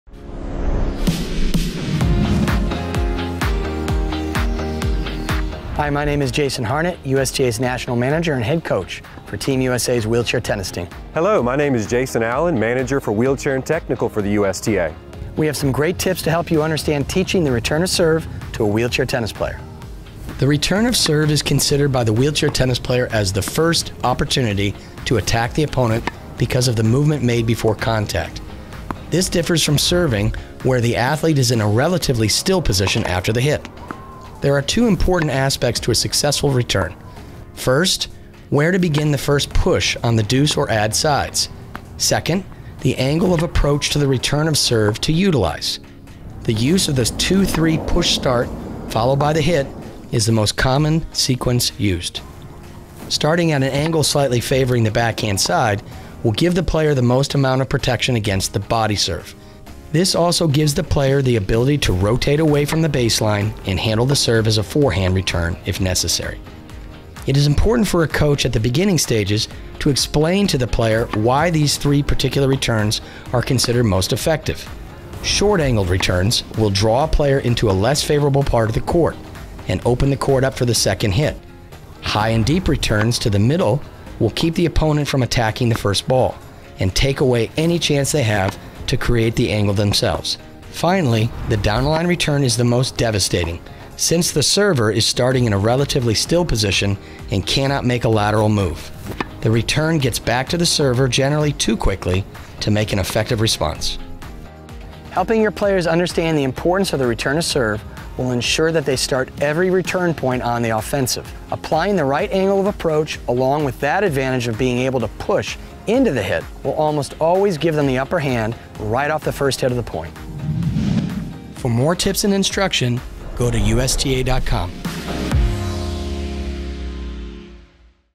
Wheelchair Improve Your Game: Return of Serve
Jason Harnett, USTA’s National Manager and Head Coach for Team USA’s Wheelchair Tennis Team, shares some great tips to help you understand the teaching aspect of the return of serve to an athlete with a disability who plays tennis in a wheelchair.